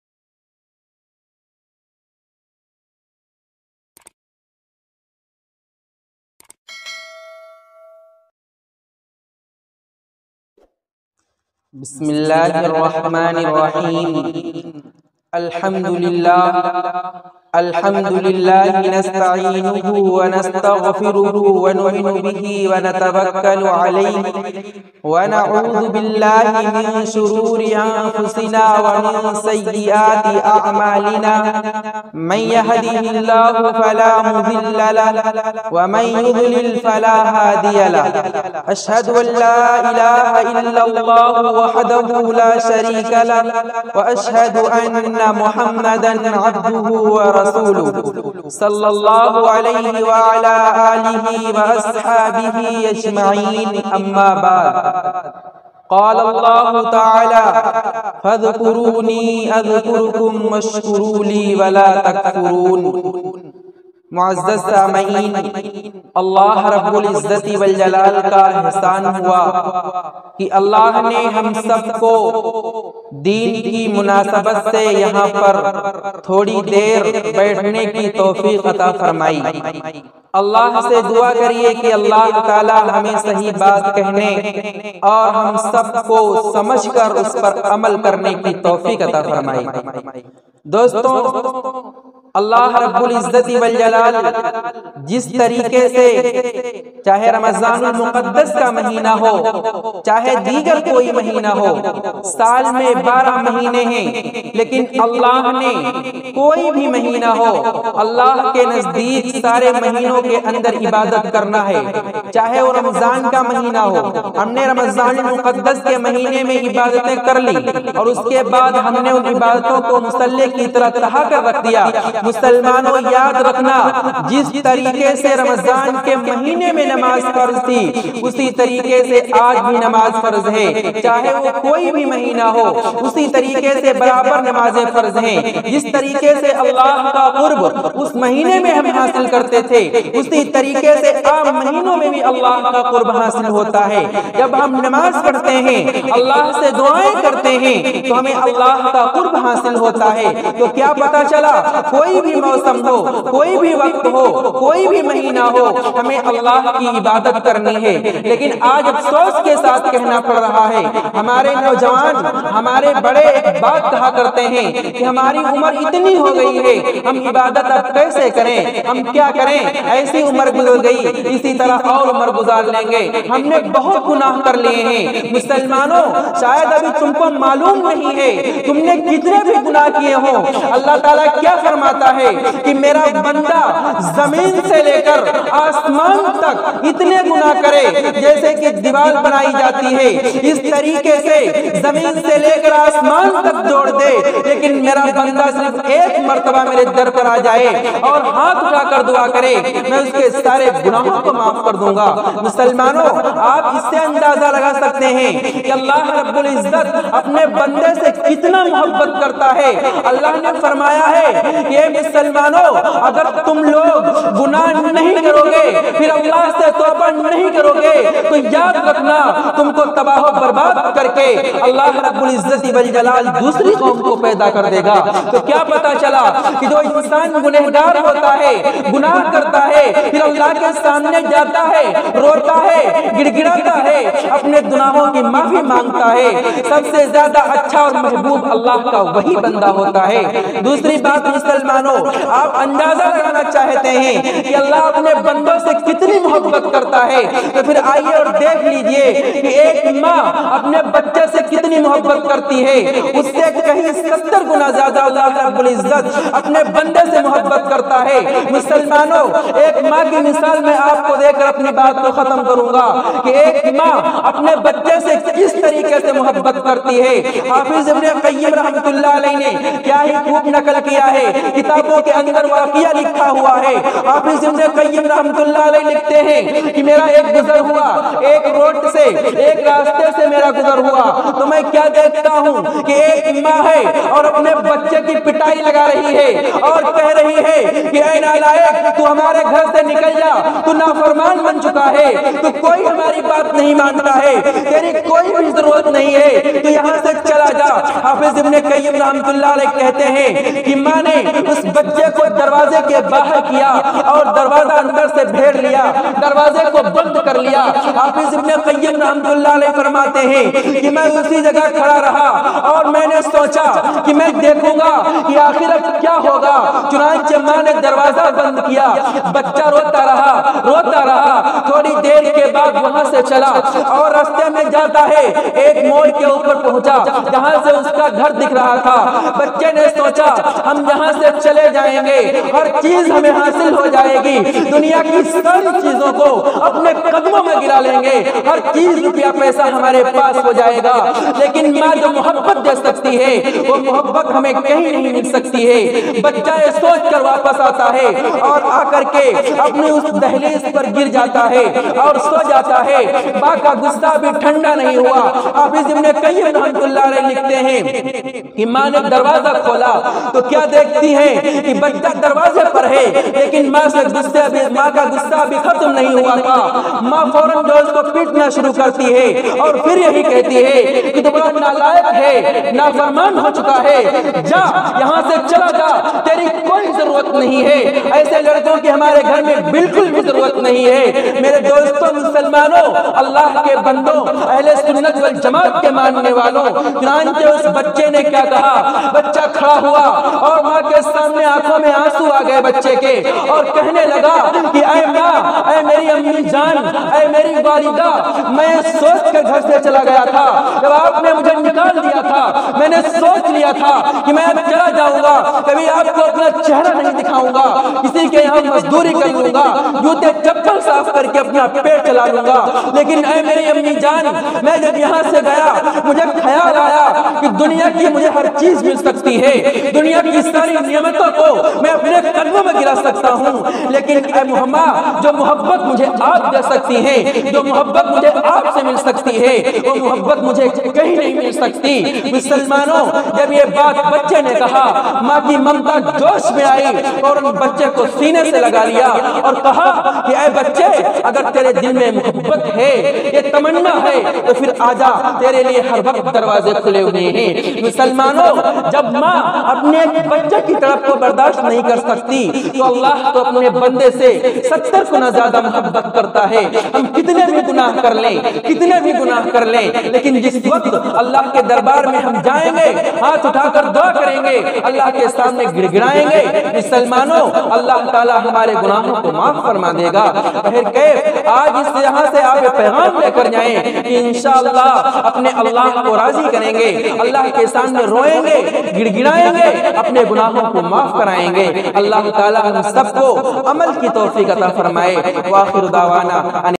Taqreer || जलसा || gunahon se taoba kaise karen By Maulana Israfil qasmi
Jalsa islahe muashra

جلسہ اصلاح معاشرہ

تقریر

तकरीर

जलसा

Bayan: Molana Israfeel qasmi
ustad madarsa Rouzatl abra deuriya